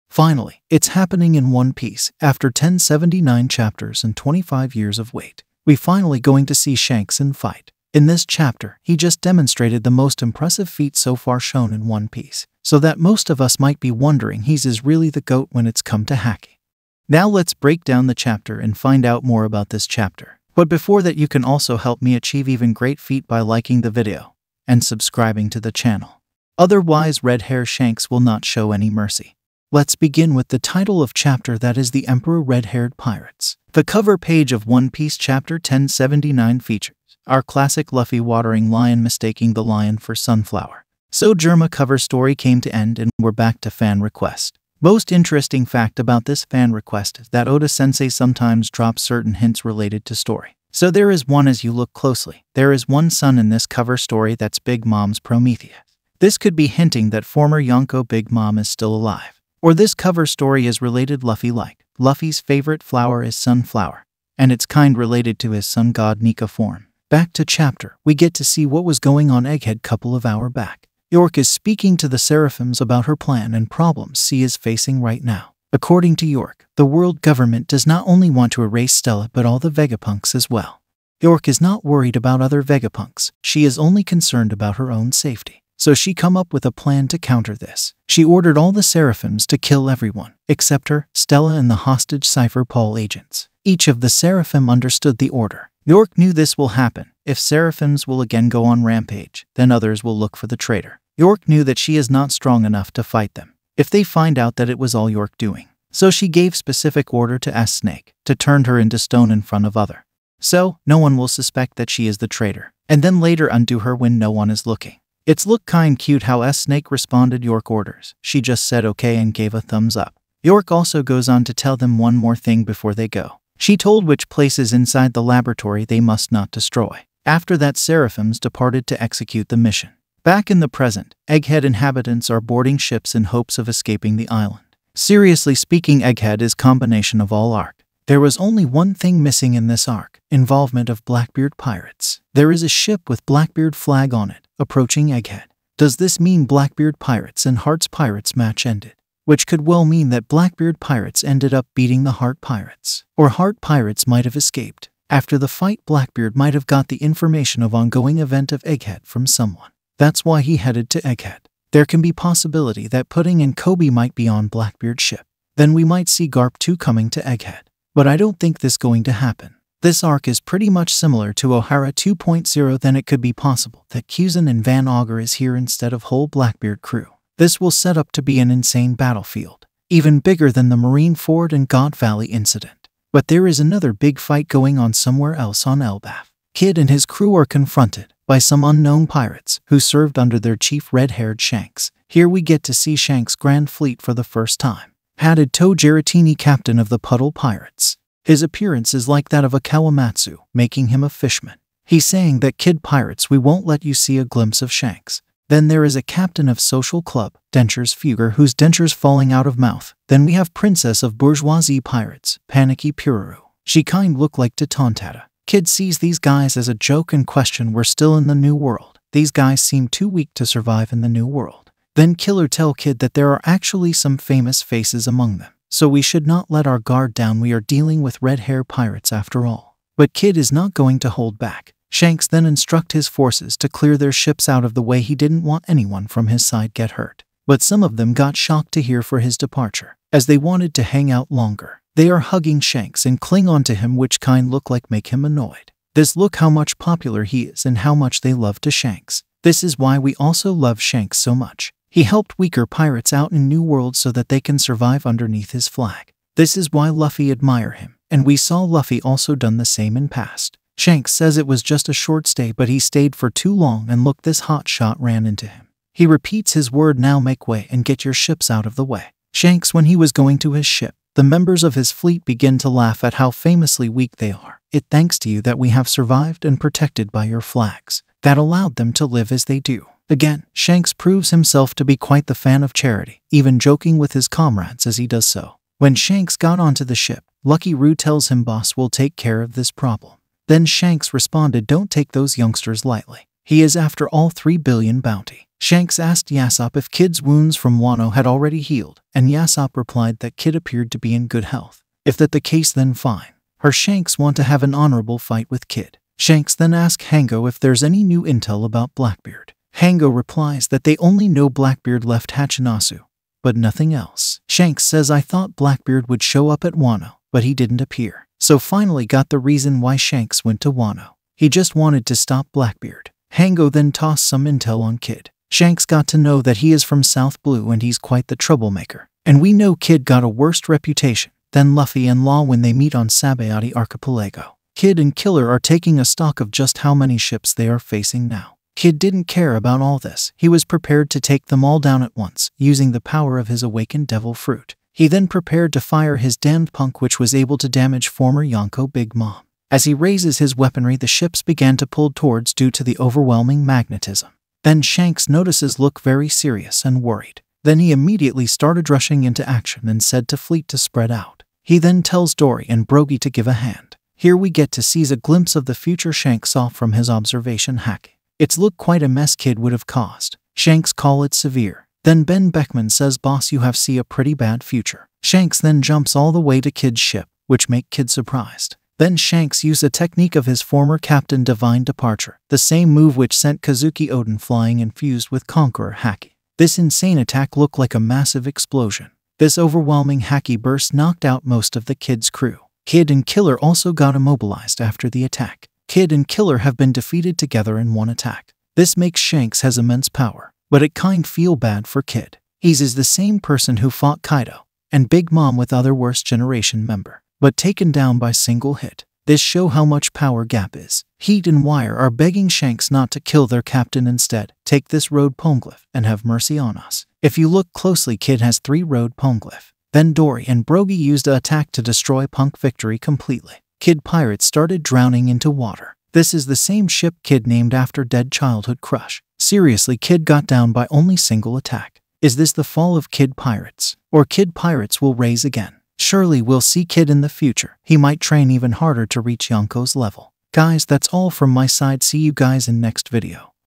This is not my end I will rise again! | One Piece Chapter 1079
Shanks defeated Eustass Kid | One piece chapter 1079 Explained.
Shanks Defeated Kid & Killer One Shot.
Kid and Killer taken down by Shanks.